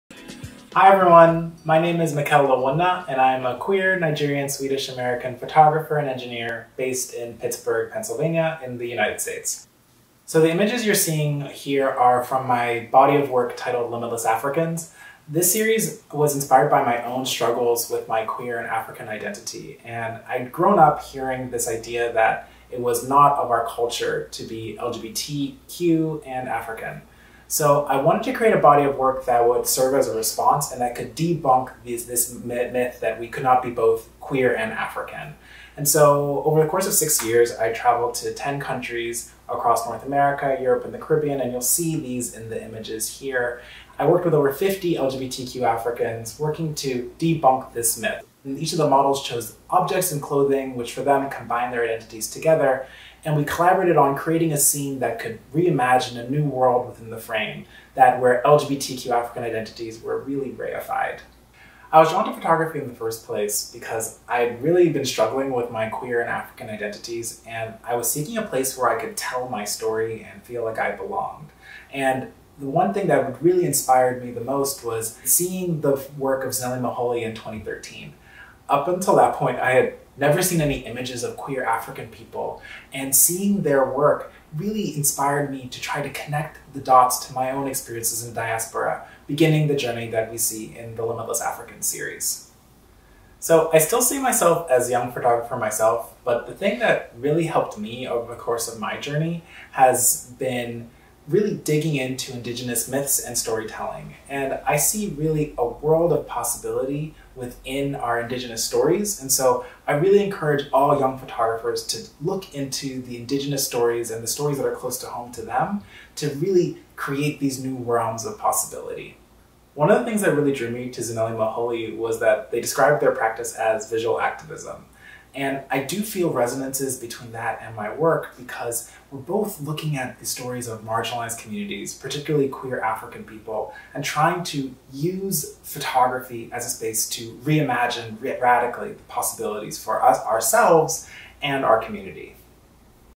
Tate Modern | Mikael Owunna
Uniqlo Tate Lates: Discussing ‘Limitless Africans’

As part of the Tate Modern exhibition of the incredible Zanele Muholi, I was invited by the Tate Modern to discuss my work and Muholi's impact, alongside several other artists including Mykki Blanco and Ka-Man Tse. This program aired on November 5, 2020.

Here is my segment in conversation with my series 'Limitless Africans.' Edited by Oli Mason.